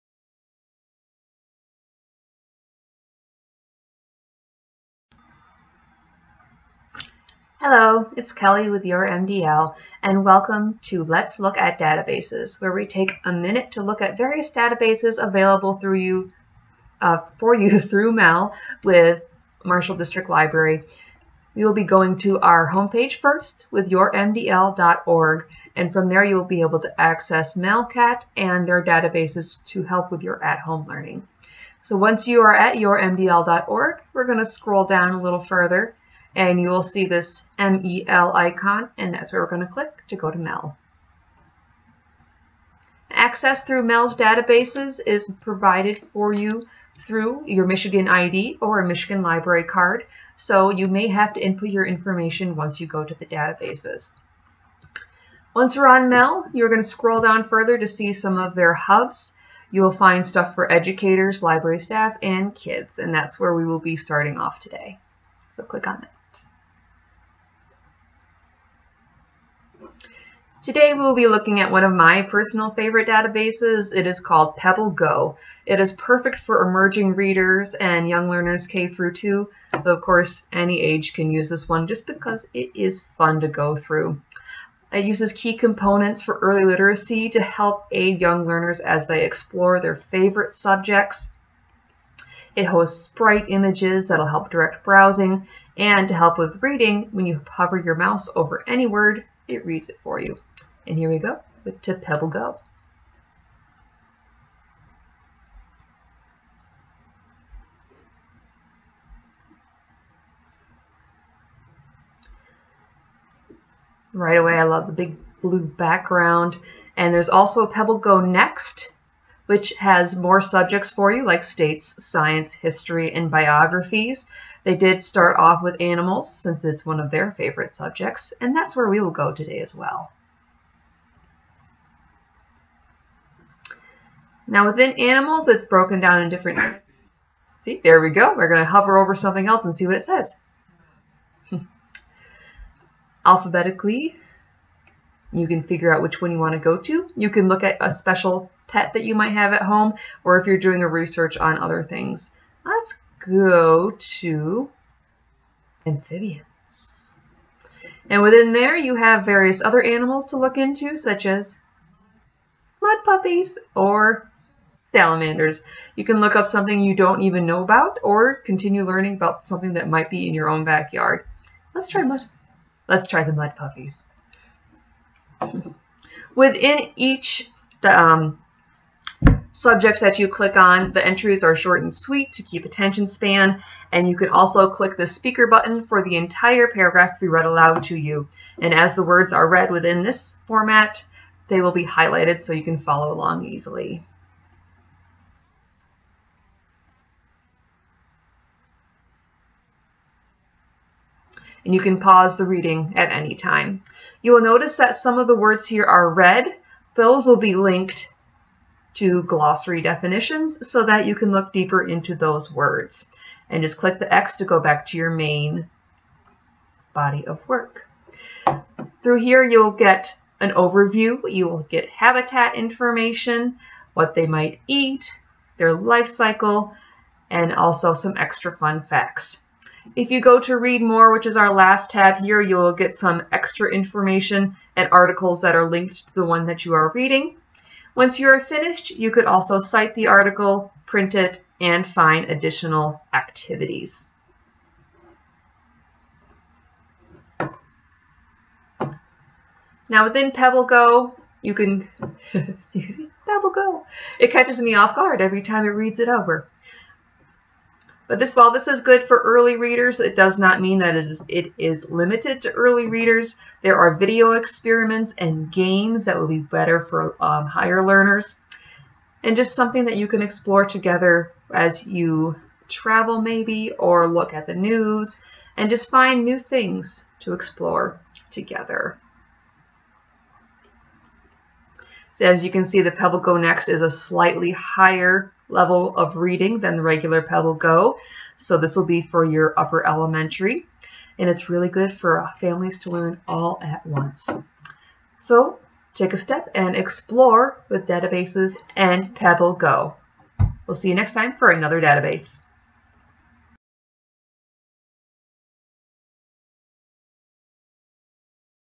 Let's Look @ Databases - Pebble Go
Let's take a look at Pebble Go, a database available through MeL, with Kelly & YourMDL.